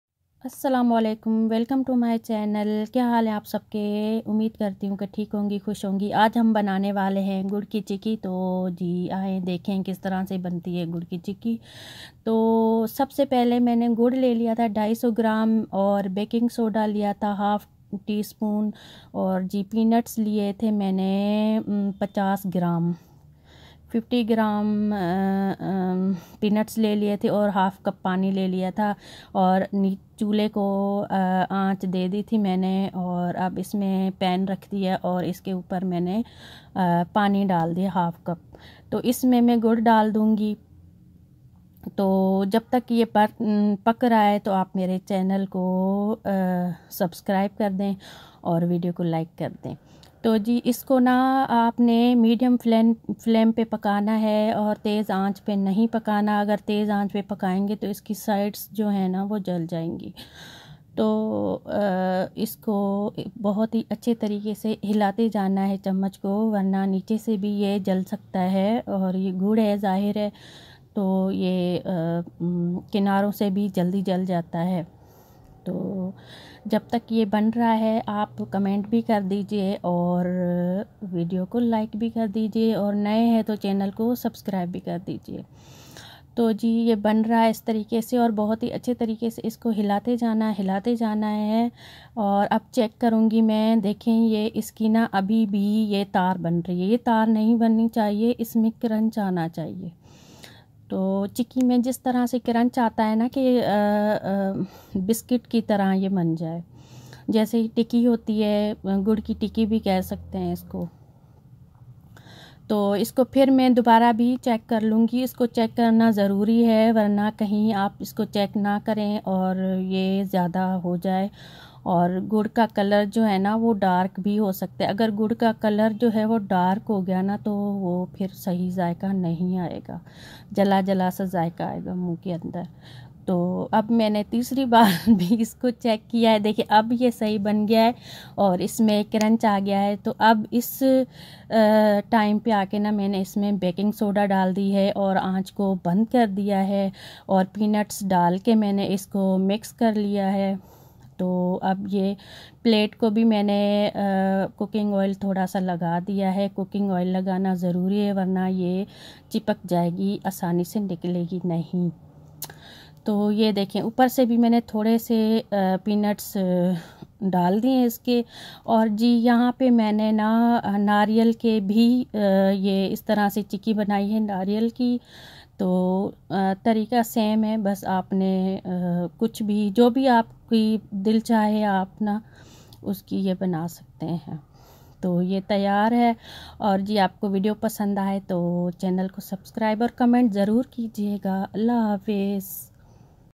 how to make gur ki chikki🤩🤤very easy method|parveennazeer20
hi guys gur ki chikki zarror try karin plz ap ko zarror passand aegi inshaAllah bohat hi easy ha banana or achi achi recipe ko dekhne k liye mere channel ko subscribe karin plz plz😍